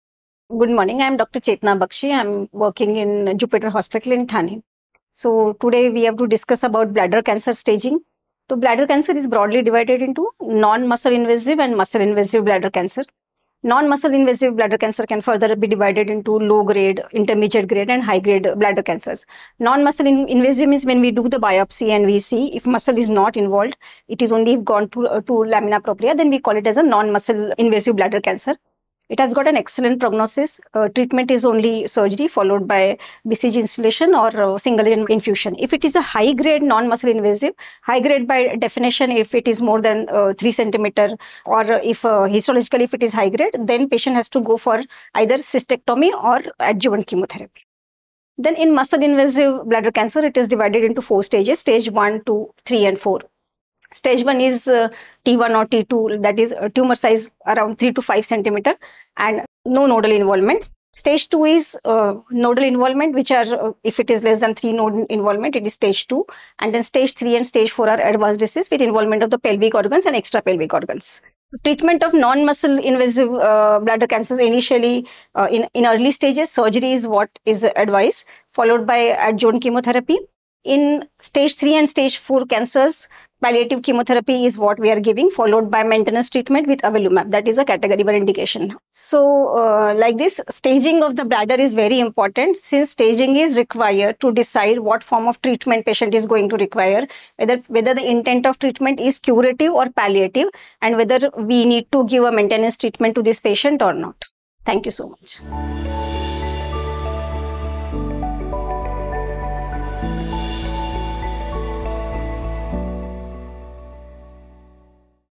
Mastering Bladder Cancer Staging with Dr. Chetna Bakshi | Bladder Cancer India
🔬 Unlocking Bladder Cancer Staging: Expert Insights with Dr. Chetna Bakshi 🔬

Welcome to Bladder Cancer India's channel! In this exclusive interview, renowned Medical Oncologist Dr. Chetna Bakshi from Jupiter Hospital Thane dives deep into Bladder Cancer Staging. Gain comprehensive knowledge about the stages, prognosis, and personalized treatment options.

🎓 About Dr. Chetna Bakshi:
Dr. Chetna Bakshi is a distinguished Medical Oncologist with a wealth of experience in treating bladder cancer. Her expertise and passion for patient care shine through as she simplifies complex staging concepts.

📺 Tune in to Bladder Cancer India's channel for more insightful discussions and updates on bladder cancer treatments, research, and patient stories.